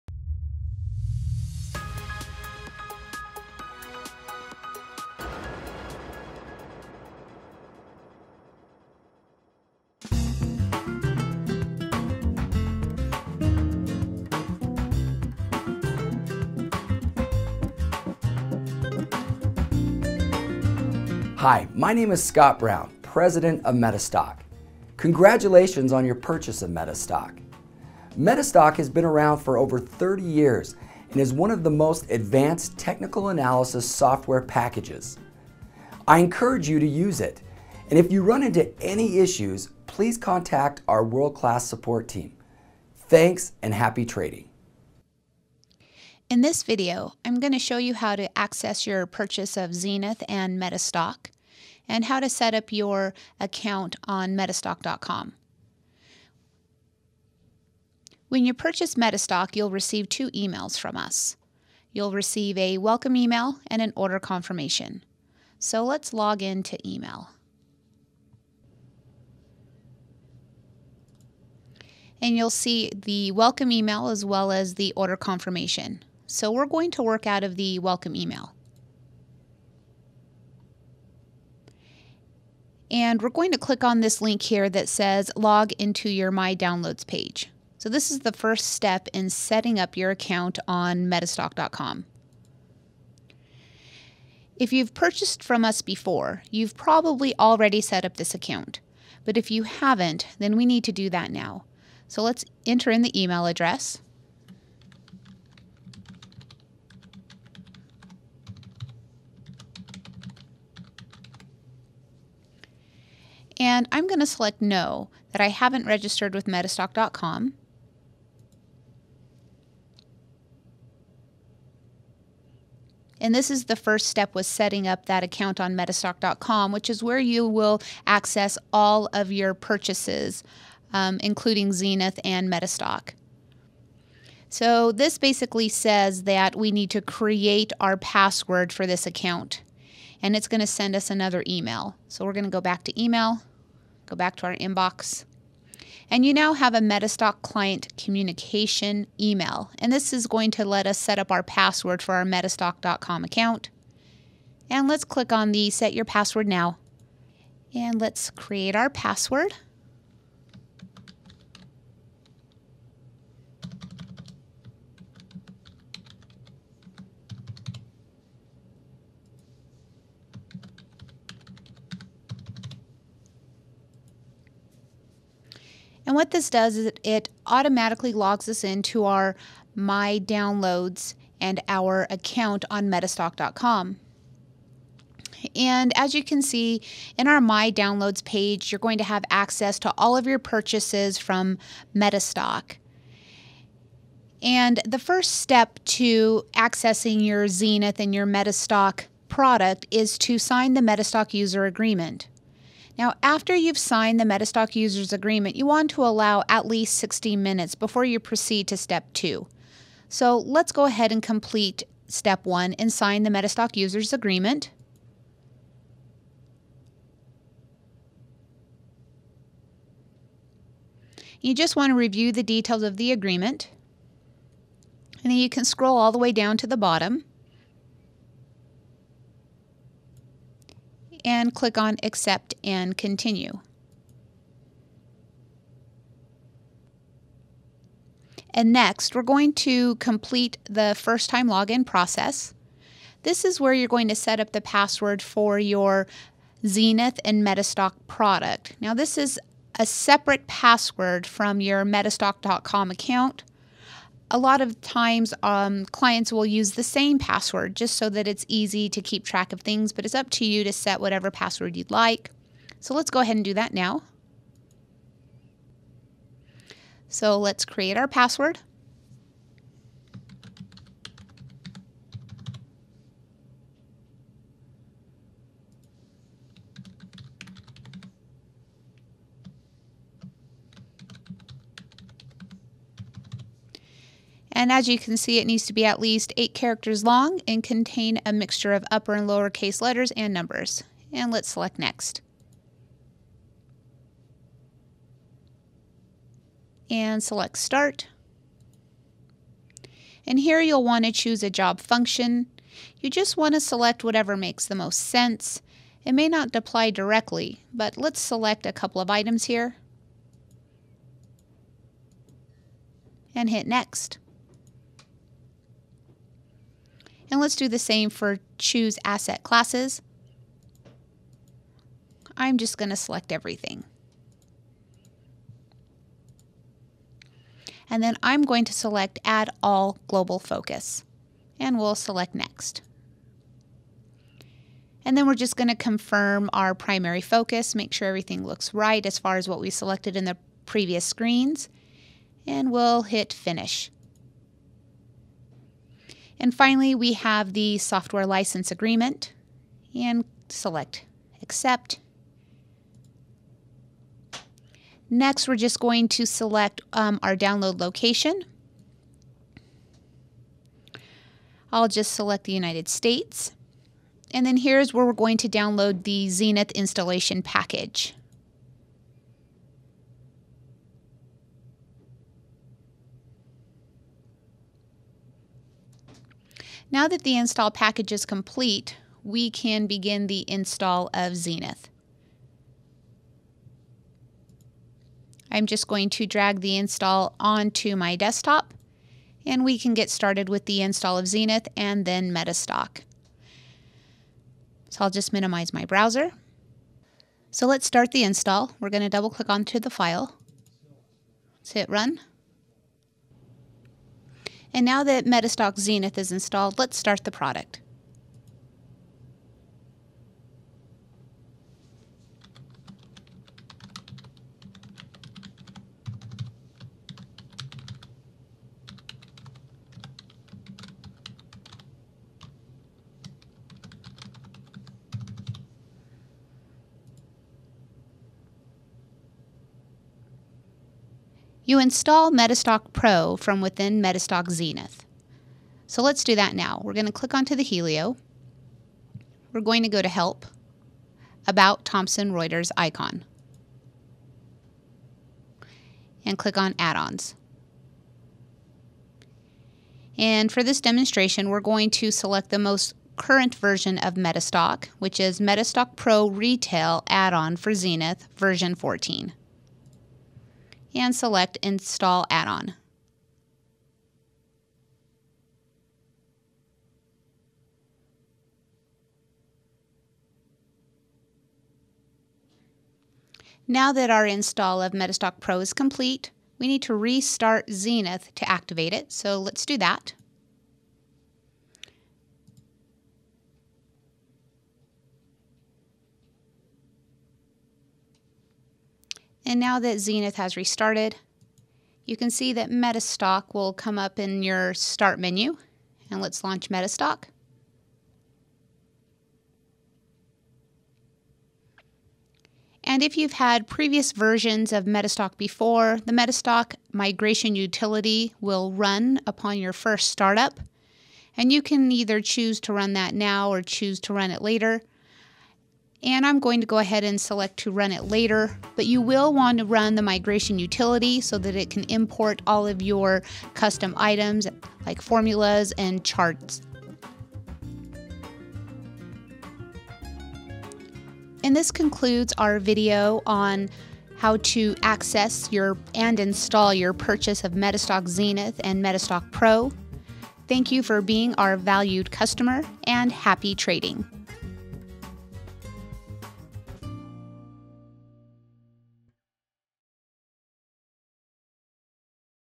Getting Started with MetaStock R/T
In the brief video Stephanie Fahrner, Director of MetaStock Support, describes the installation process of XENITH and MetaStock R/T.